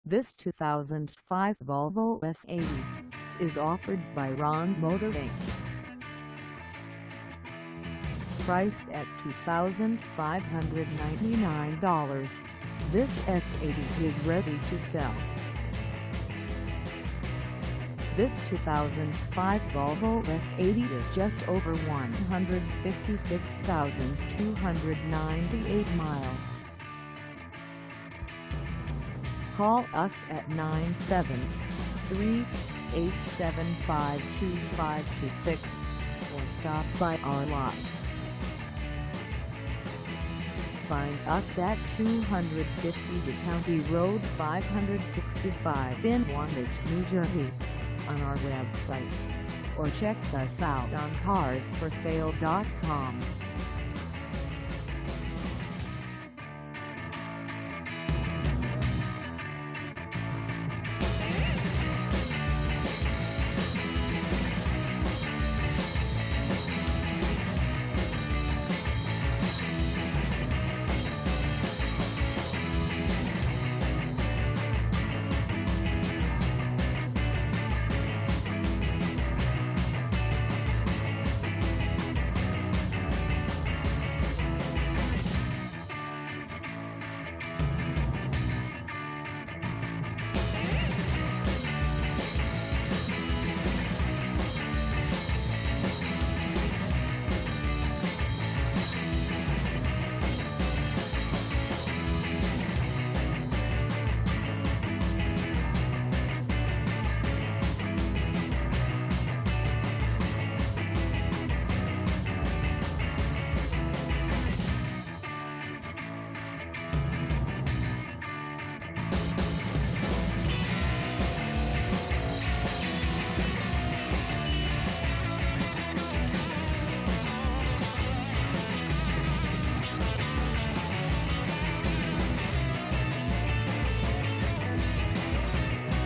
2005 Volvo S80 2.5T 4dr Turbo Sedan for sale in Wantage, NJ
This 2005 Volvo S80 2.5T 4dr Turbo Sedan is for sale in Wantage, NJ 07461 at Ron Motor Inc..

Engine: 2.5L I5 Turbocharger
Transmission: Automatic 5-Speed
Color: Black
Mileage: 156298
Contact Ron Motor Inc. 252 County Rd 565 Wantage, NJ 07461 to test drive this 2005 Volvo S80 2.5T 4dr Turbo Sedan today.